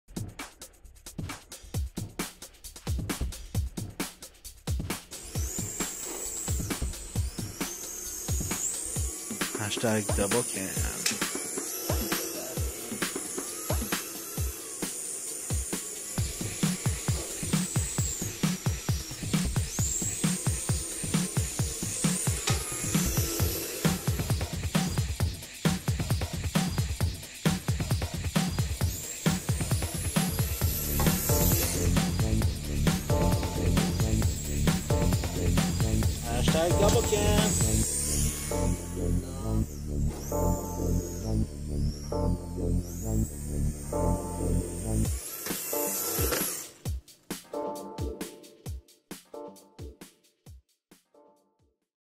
DoubleCan - RC Drift popcan trick and outtakes HD
Happy New Year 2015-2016 from RockitRC! Enjoy the fun clip ;-)

Music programmed by RockIt RC via Launchpad by Novation
Filmed with a Samsung S4